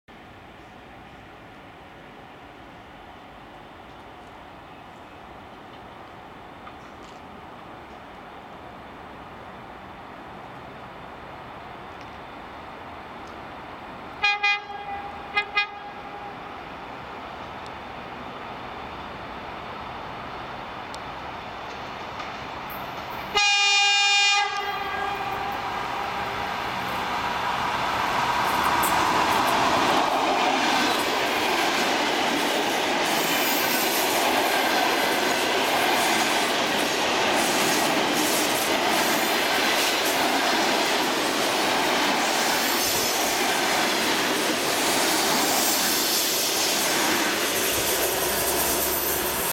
Электропоезд ЭД4м-0489
Электропоезд ЭД4м-0489, пл.Савеловская, Москва